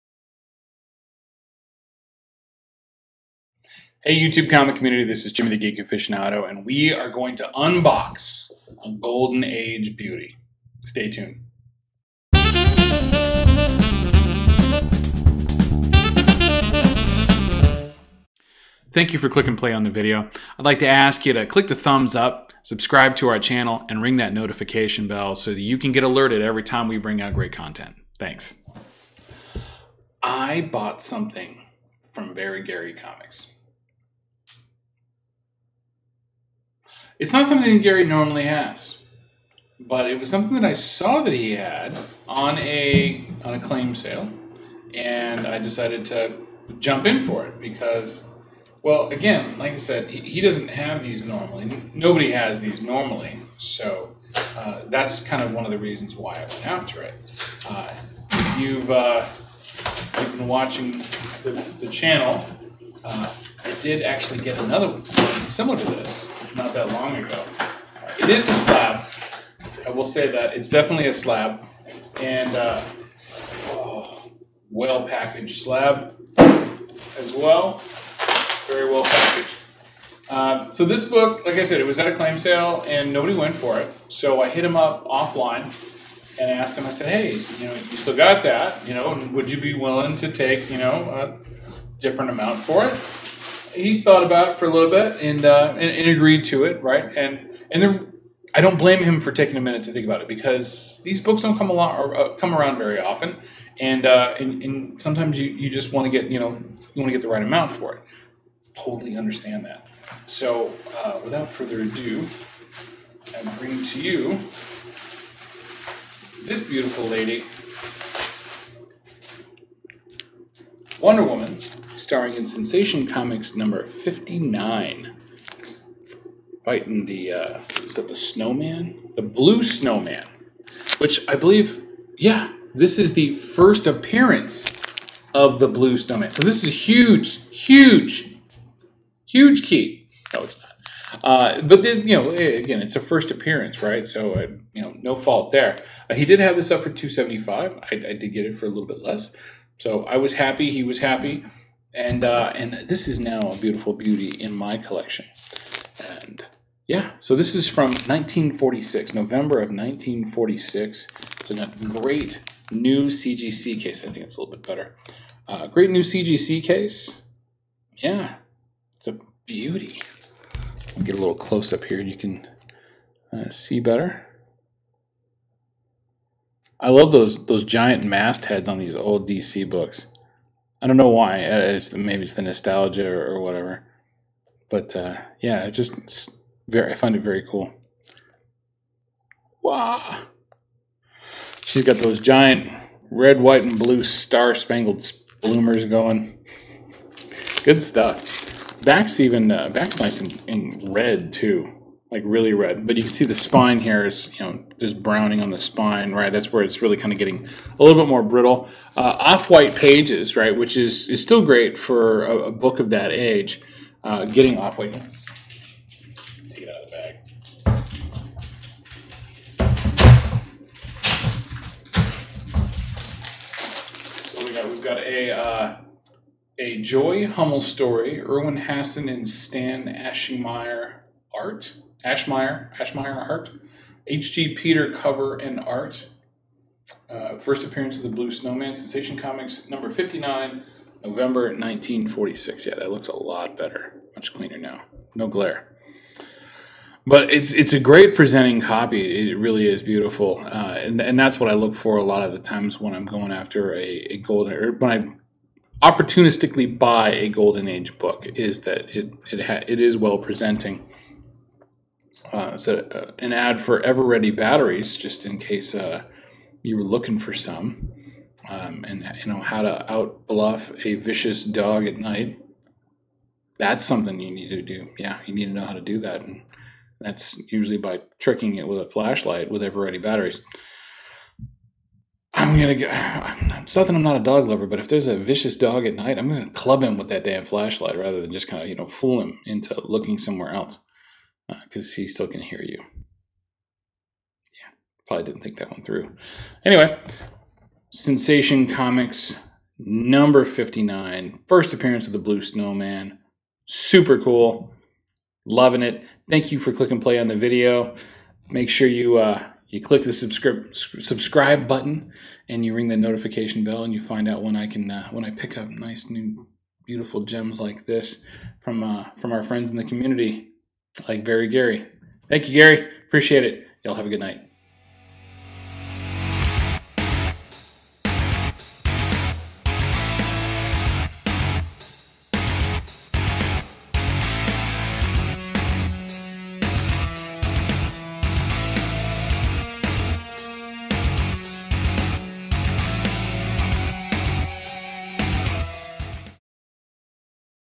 Unboxing A Golden Age Goodie from Very Gary Comics!
I got this incredible golden age book from our community friend, Very Gary Comics! Check out the reveal in this video.

FB Messenger: m.me/thegeekaficionado